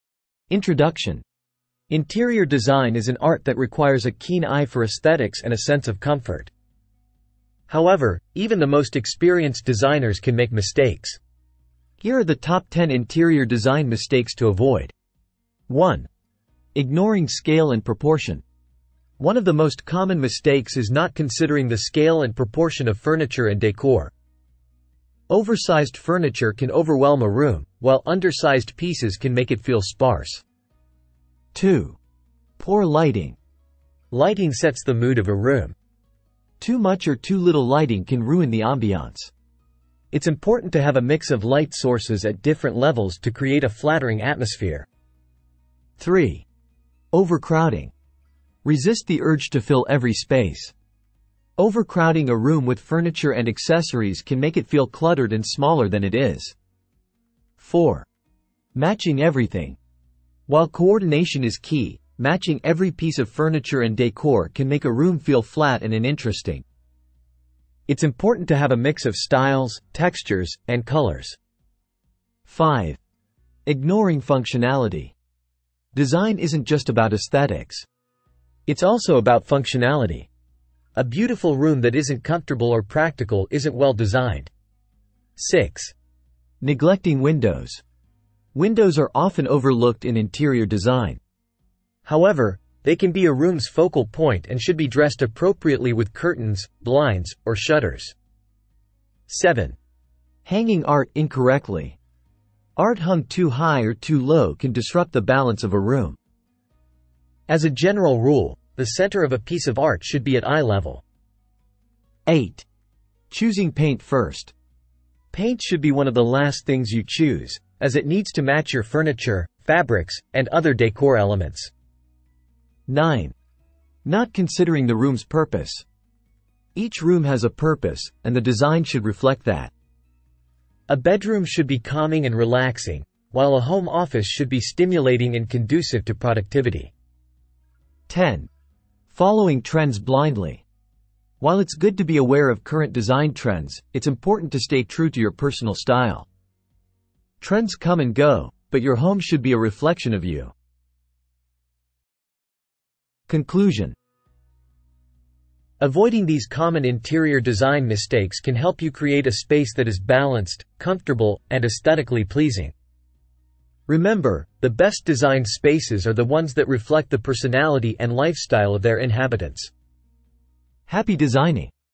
Top 10 Interior Design Mistakes to Avoid
Navigate the world of interior design with confidence by avoiding these top 10 common mistakes. From neglecting proper lighting to overcrowding spaces, learn how to sidestep pitfalls and create harmonious and visually appealing interiors.